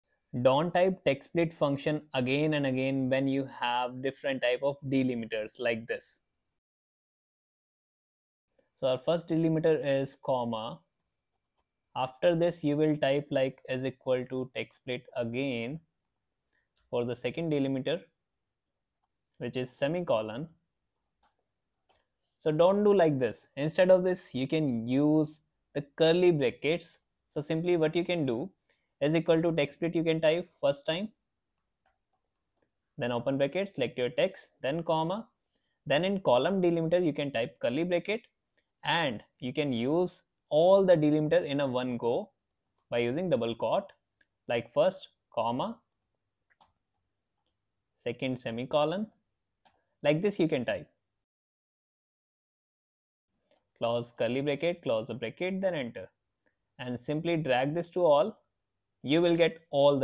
Don't Use Textsplit in Excel like this‼️Instead Use Amazing Trick
In this video, you will learn how to use textsplit function with different delimiters in Excel.